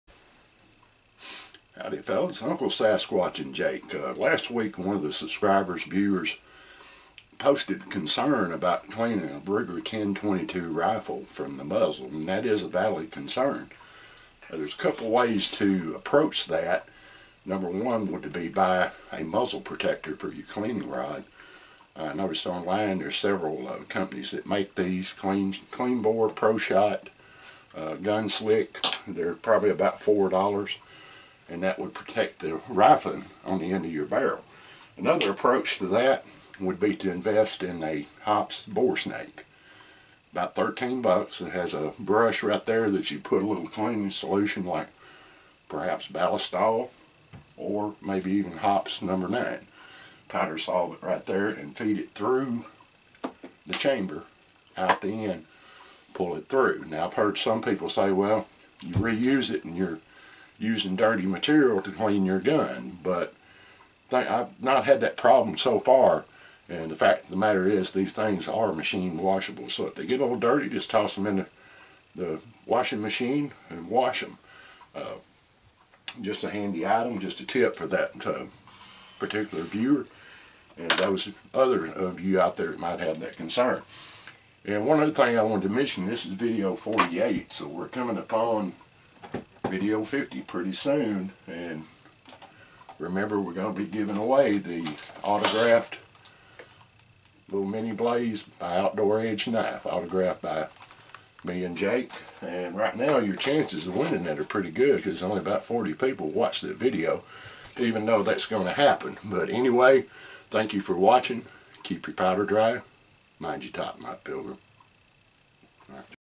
Ruger 10/22 Cleaning Methods
This video is a reponse to a viewer's concerns over cleaning from the muzzle.